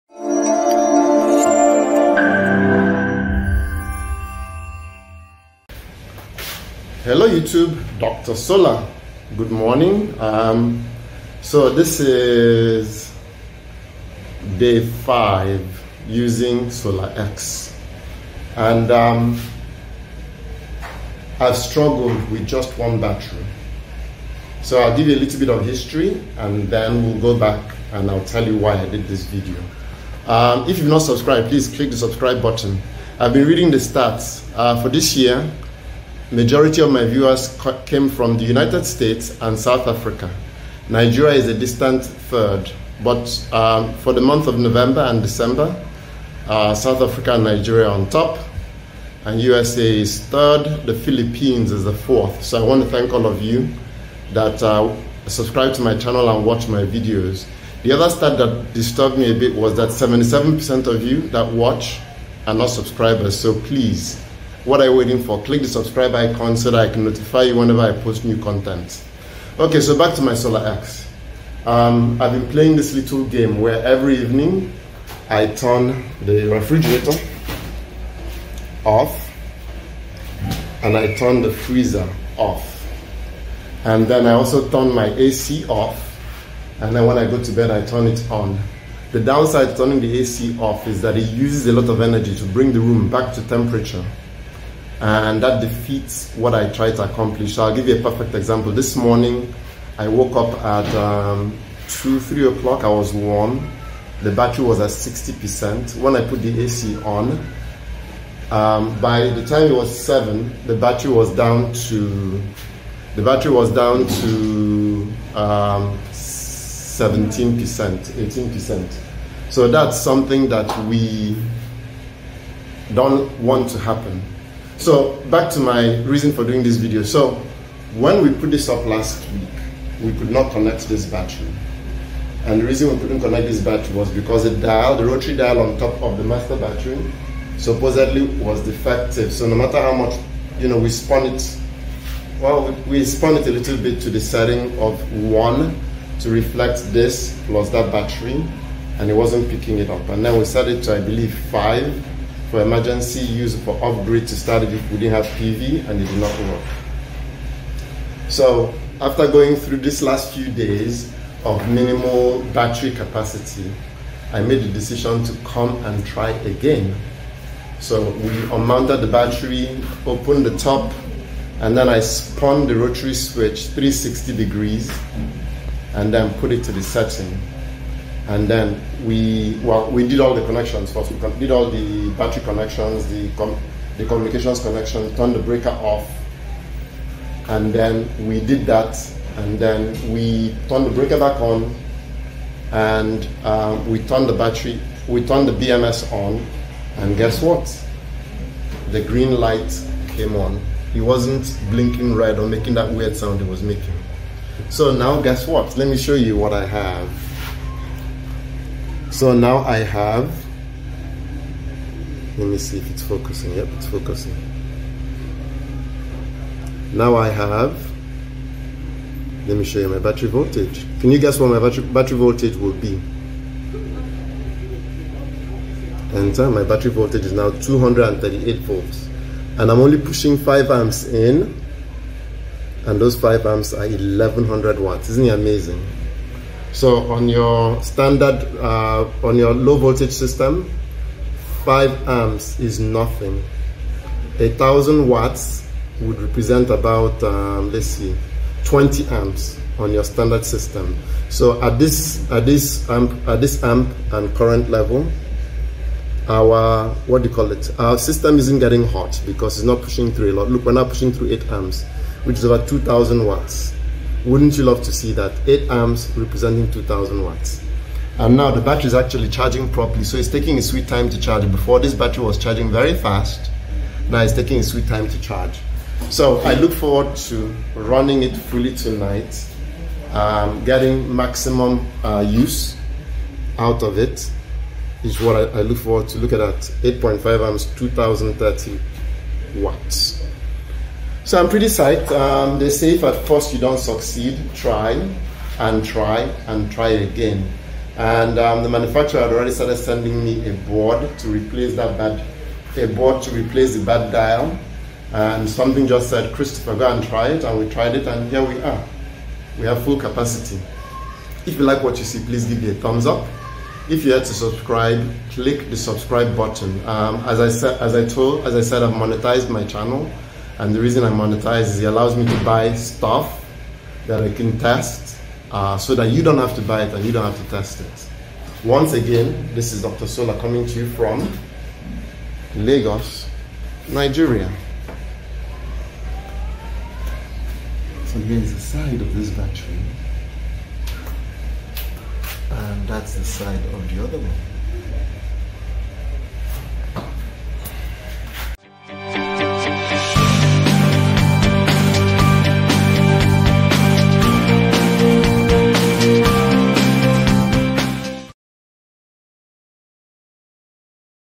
If at first you don't succeed try and try again. SolaX Triple Power Battery revived.
I finally got the 2nd SolaX battery to work. It was a no brainer